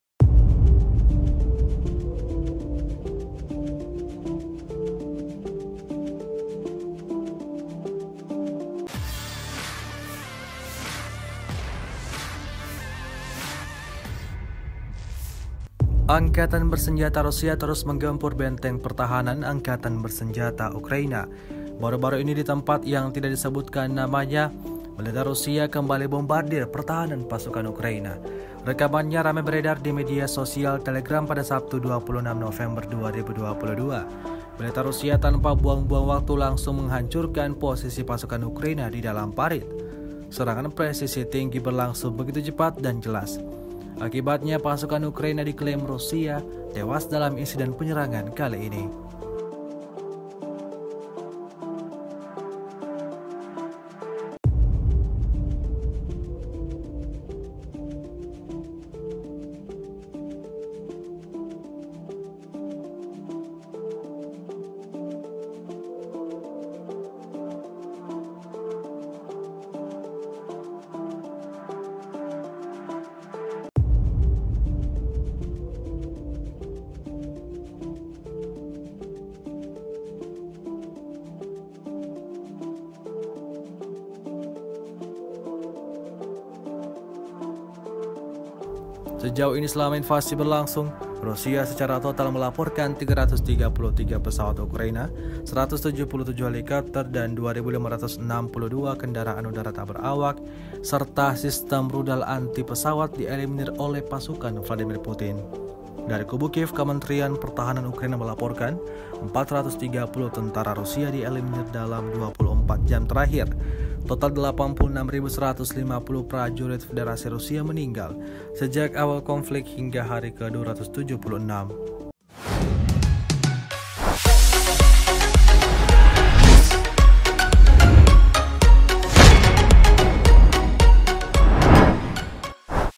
Ledakan Dahsyat di Parit Pasukan Ukraina, Ulah Tentara Putin!
TRIBUN-TIMUR.COM - Angkatan Bersenjata Rusia terus menggempur benteng pertahanan Angkatan Bersenjata Ukraina.

Baru-baru ini di tempat yang tidak disebutkan namanya, militer Rusia kembali bombardir pertahanan pasukan Ukraina.

Militer Rusia tanpa buang-buang waktu langsung menghancurkan posisi pasukan Ukraina di dalam parit.

Serangan presisi tinggi berlangsung begitu cepat dan jelas.

Akibatnya, pasukan Ukraina diklaim Rusia tewas dalam insiden penyerangan kali ini.

Rusia melaporkan secara total, 333 pesawat Ukraina dan 177 helikopter, 2.562 kendaraan udara tak berawak, 390 sistem rudal anti-pesawat dieliminir sejak awal operasi khusus.

Narator: Memet